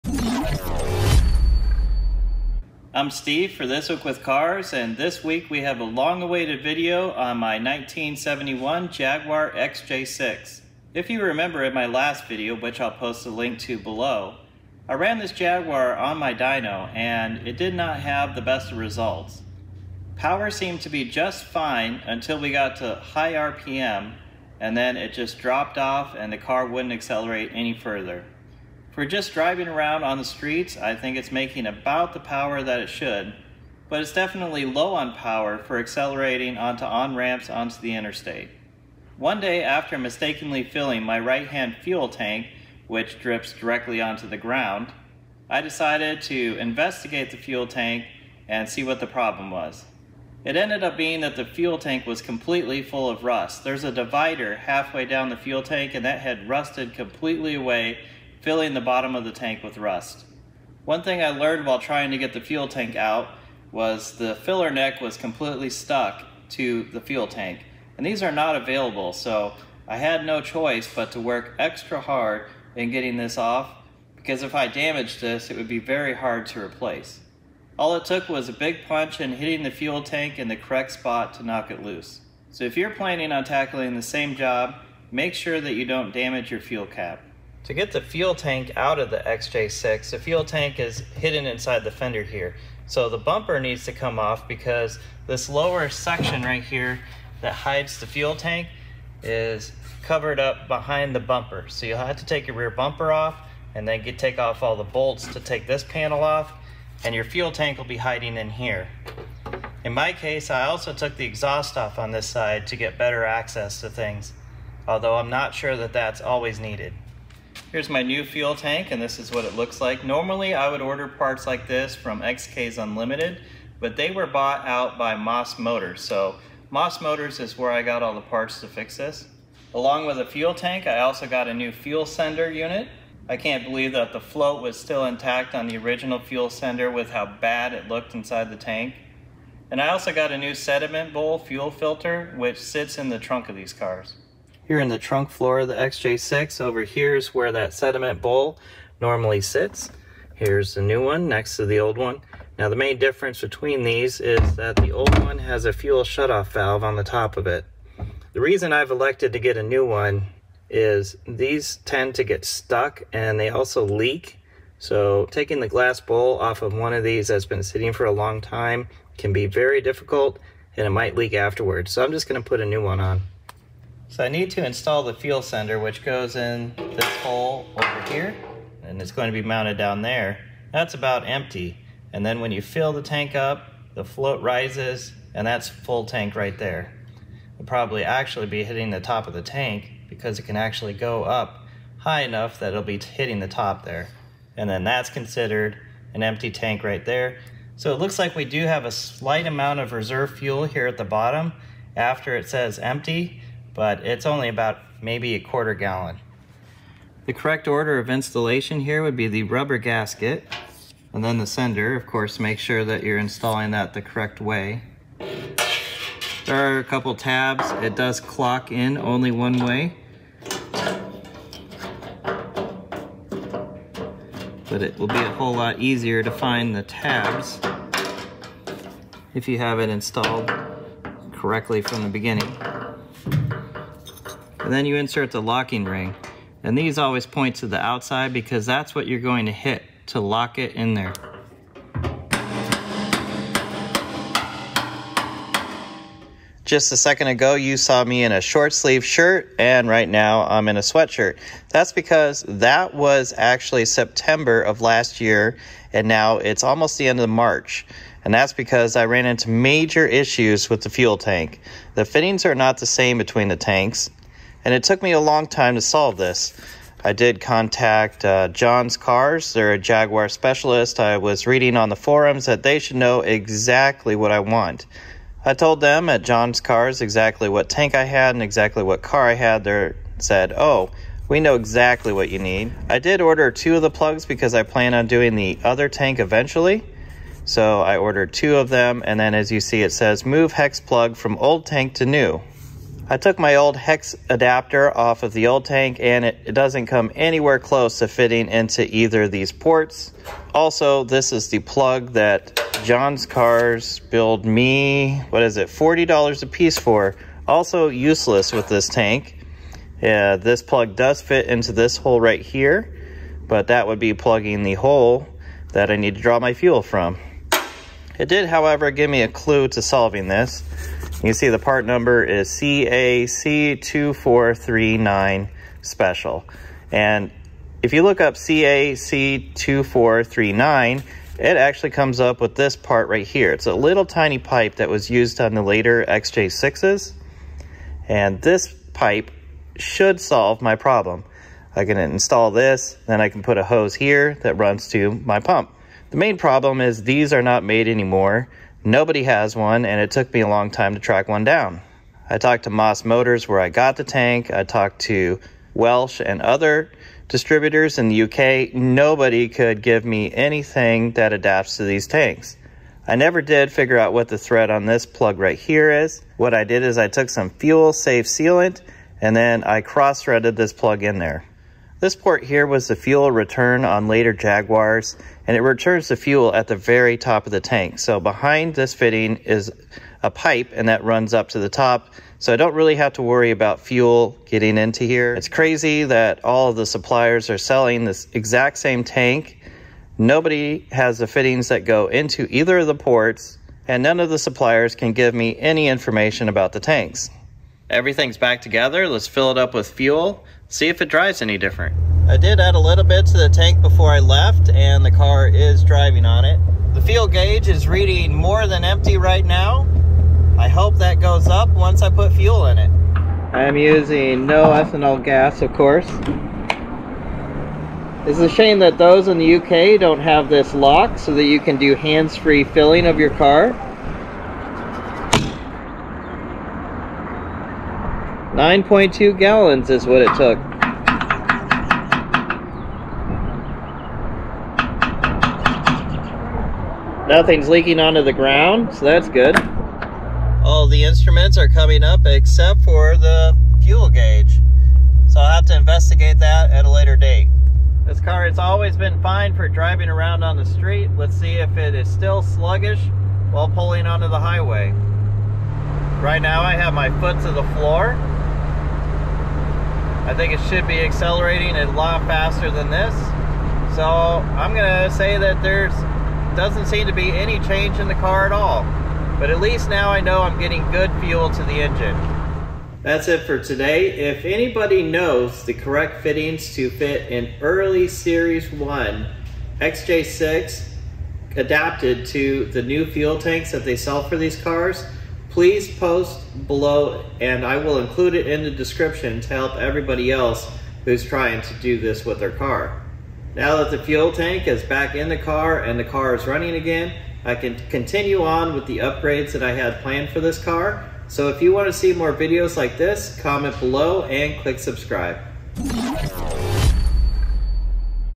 Replacing Ruined Fuel Tank on a Jaguar XJ6
Today I replace the right side fuel tank and sediment bowl in my Series I Jaguar XJ6.  The fittings on the new fuel tank did not match the tank which derailed this project for months.  Watch my last video where I ran this car on the dyno to disappointing results